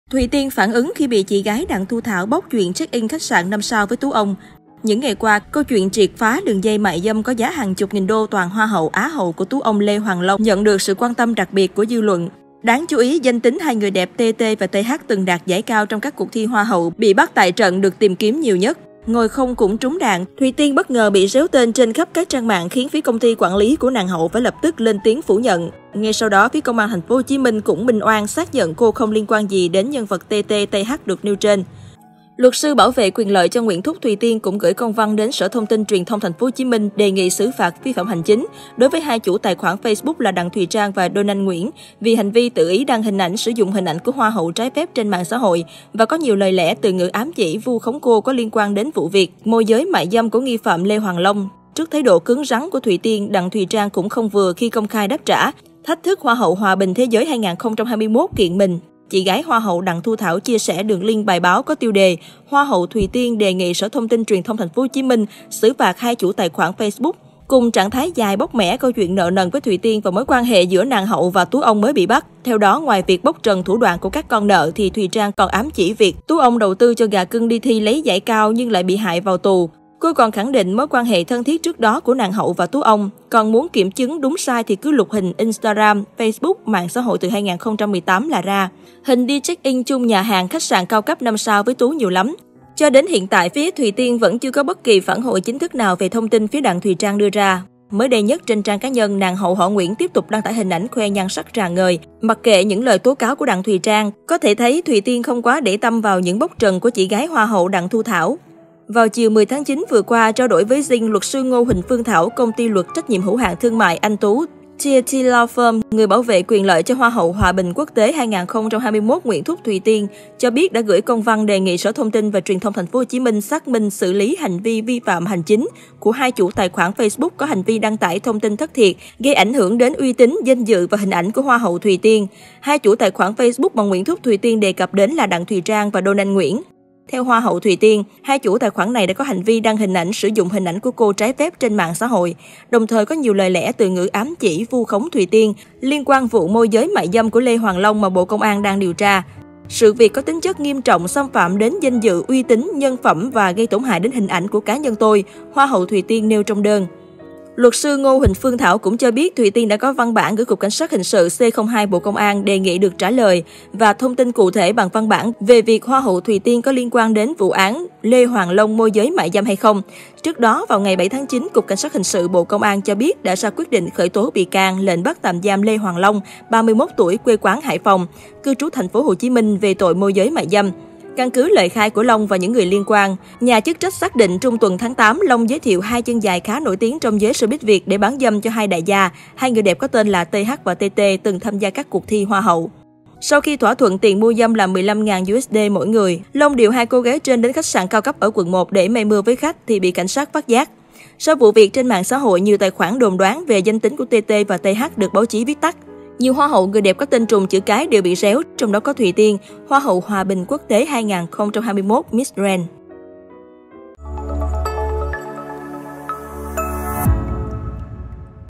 Thùy Tiên phản ứng khi bị chị gái Đặng Thu Thảo ‘bóc’ chuyện check in khách sạn 5 sao với ‘tú ông’
Những ngày qua, câu chuyện triệt quá đường dây mại dâm có giá hàng chục nghìn đô toàn Hoa hậu, Á hậu của "tú ông" Lê Hoàng Long nhận được sự quan tâm đặc biệt của dư luận. Đáng chú ý, danh tính 2 người đẹp T.T và T.H từng đạt giải cao trong các cuộc thi Hoa hậu bị bắt tại trận được tìm kiếm nhiều nhất.
Ngồi không cũng trúng đạn, Thùy Tiên bất ngờ bị réo tên trên khắp các trang mạng khiến phía công ty quản lý của nàng hậu phải lập tức lên tiếng phủ nhận. Ngay sau đó, phía Công an TP.HCM cũng "minh oan", xác nhận cô không liên quan gì đến nhân vật T.T, T.H được nêu trên.